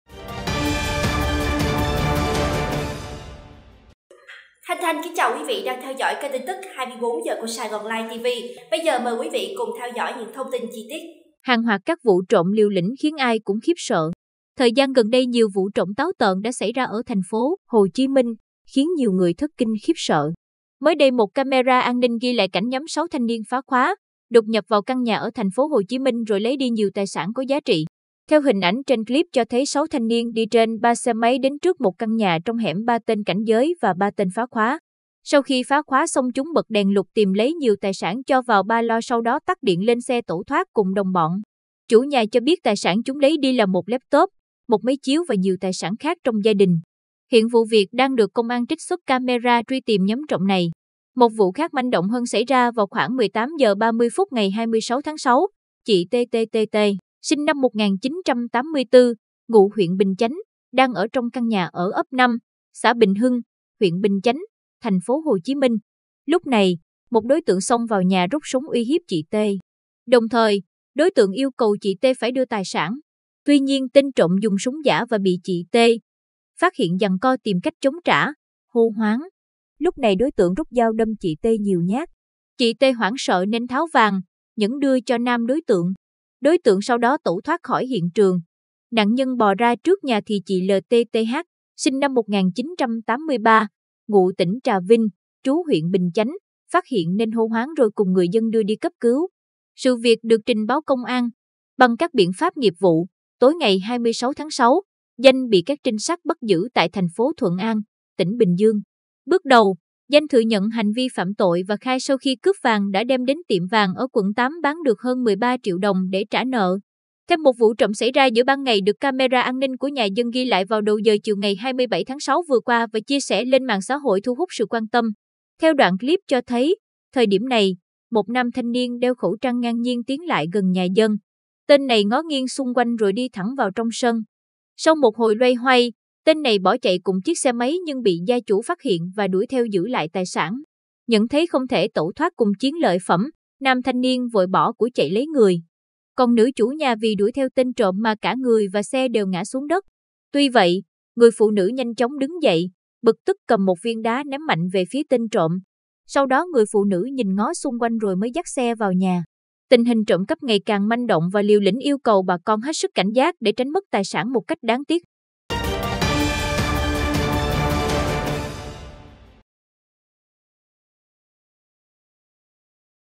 HÀNG HOẠT CÁC VỤ TRỘM LIỀU LĨNH KHIẾN AI CŨNG KHIẾP SỢ | TIN TỨC 24H | SAIGON LIFE TV
Thời gian gần đây nhiều vụ trộm táo tợn đã xảy ra ở Tp. HCM, khiến nhiều người thất kinh khiếp sợ.